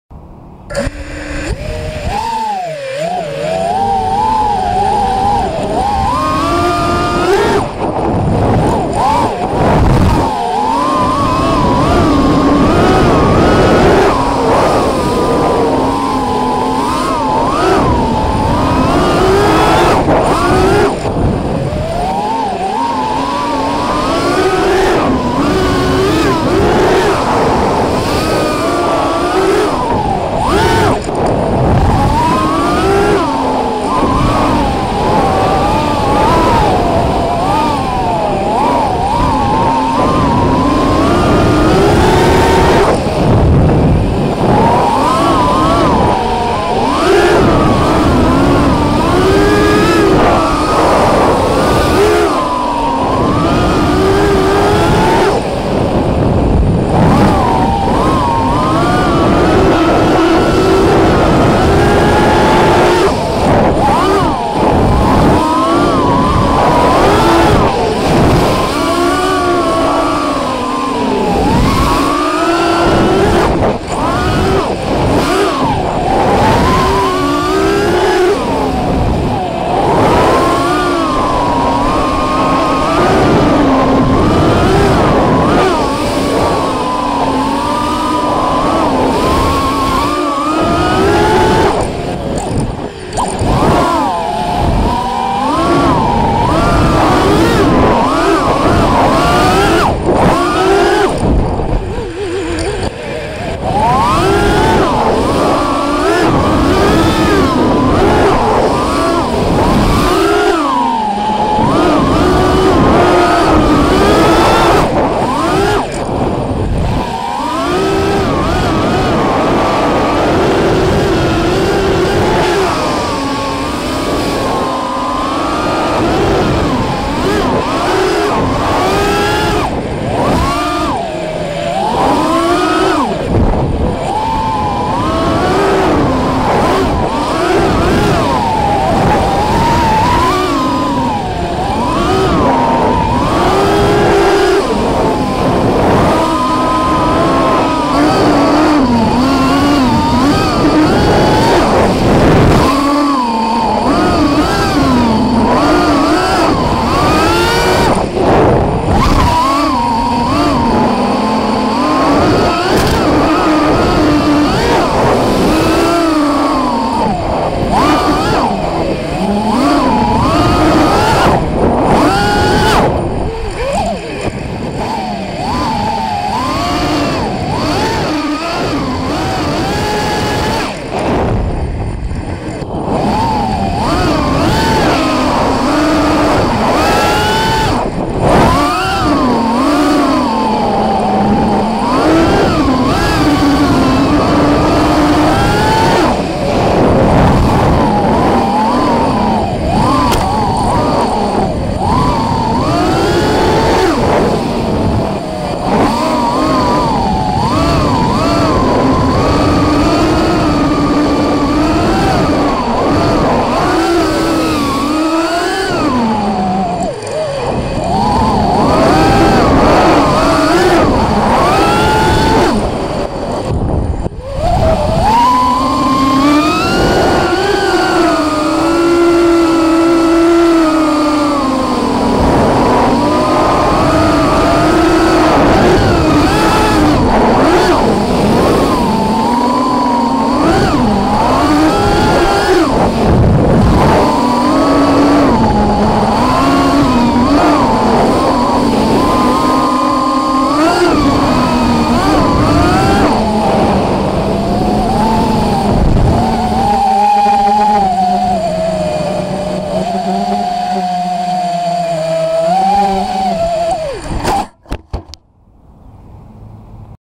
Warping through the tree!
Need for speed anyone? This wasn't my location of choice. I drove to a few spots but guess what? Regardless of 43 Degrees Celsius (110 degrees F) fire blazing sun oh and nearly 60% humidity, there were too many people up in all the nice spots.

The sun "scorched" me but I guess it was all worth it, after I found this spot. There were no "human beings" around as you might see in a high aerial view of the car park and the park itself. I had plenty of fun and flew a few packs. I had some near misses with them "Ghost branches" and eating grass (I think I mowed some grass for sure).

FPV Setup
Fat Shark Goggles
VTX TBS Unify Pro HV
Foxeer Arrow V2 (2.5 mm lens)
RunCam 3

Frame

Alien 5 inch
Motors Emax Red Bottoms S
FC KISS
ESCs KISS
Props HQ 5x4x4